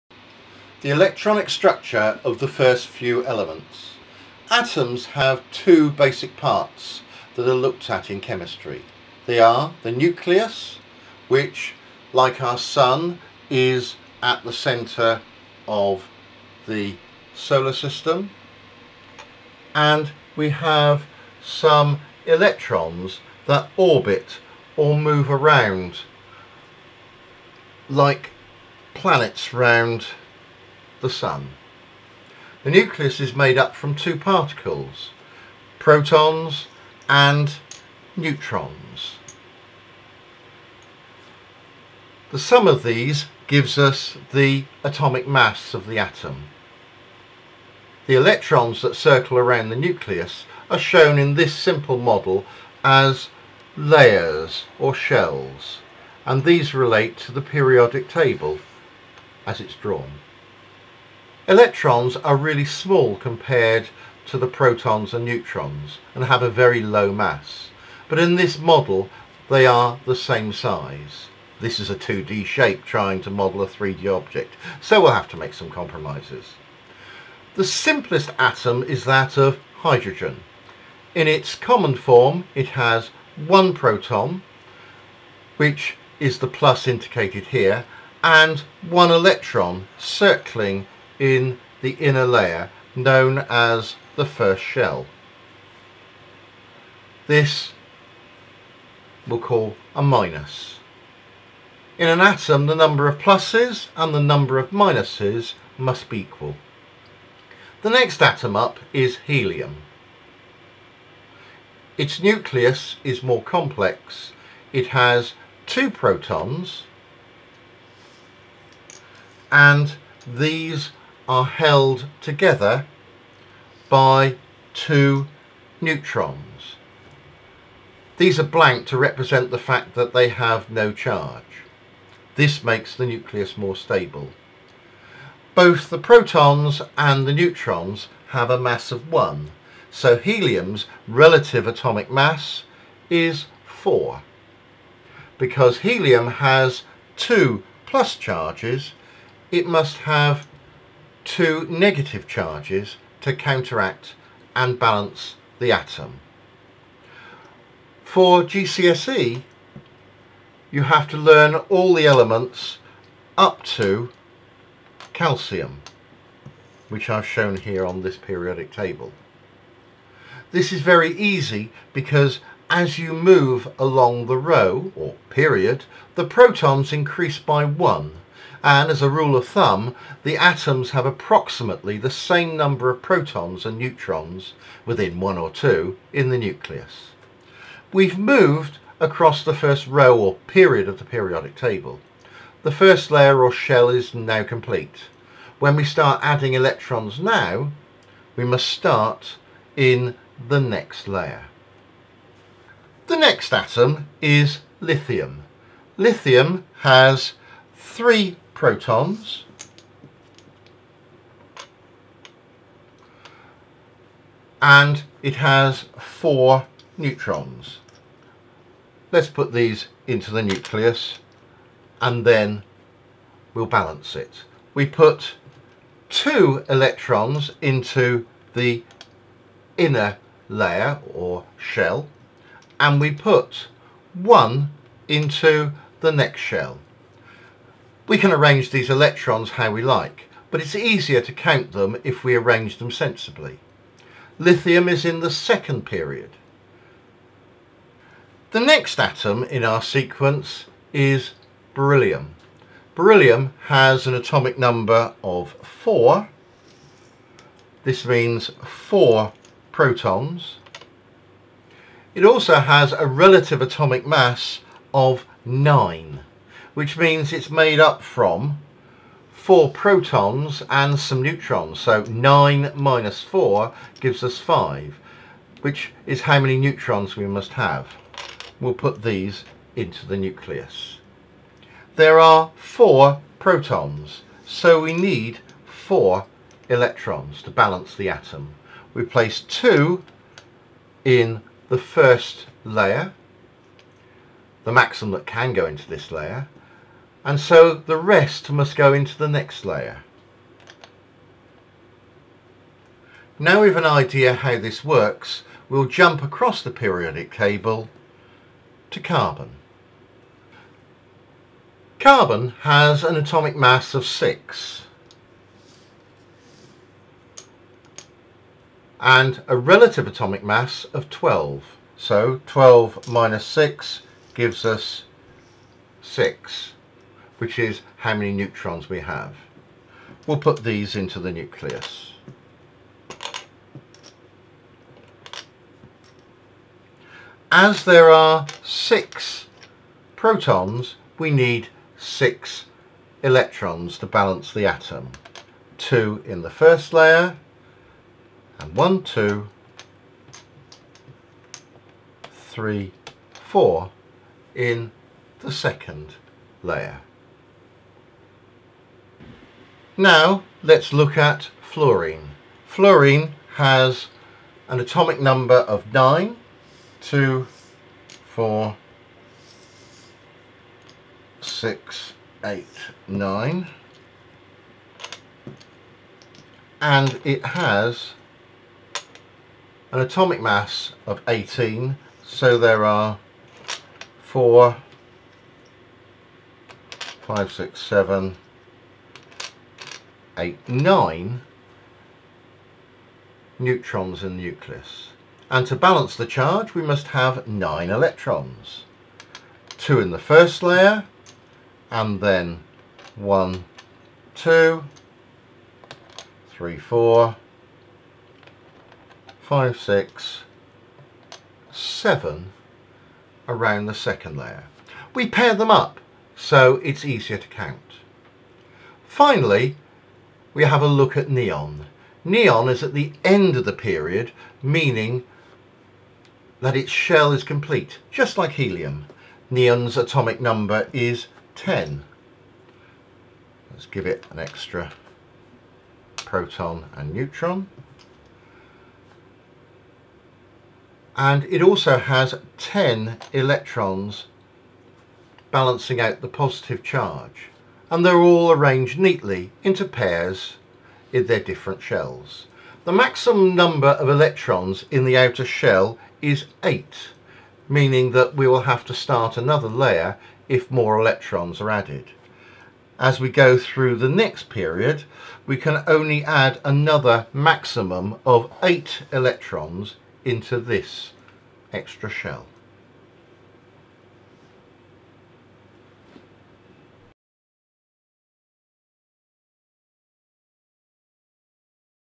Tutorial on the Electronic Structure of first few Elements of the Periodic Table
In this tutorial we are looking at the shells of atoms examining their electronic arrangement of electrons in the first and second period of the periodic table. We look at the 2,8,8 layer arrangement of shells and how we make the, We start looking at the electronic configuration of hydrogen then the electronic configuration of Helium then the Electronic configuration of Lithium and then continue through Carbon, Fluorine to go up to Neon. We look at the 2,8,8 structure and arrangements of the electrons around the atoms.